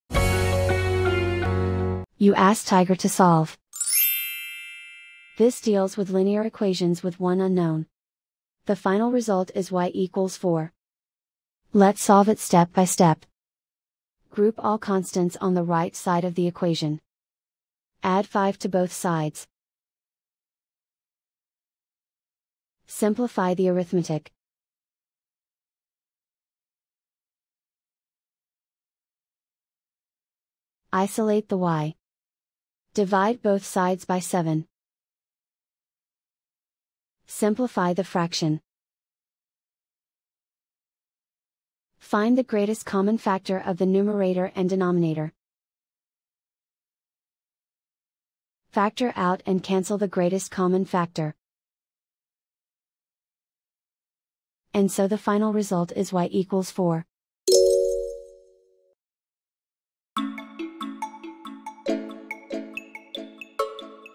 Solve 7y-5=23: Linear Equation Video Solution | Tiger Algebra
Struggling with 7y-5=23? Watch our step-by-step video on Tiger Algebra to master this linear equation effortlessly.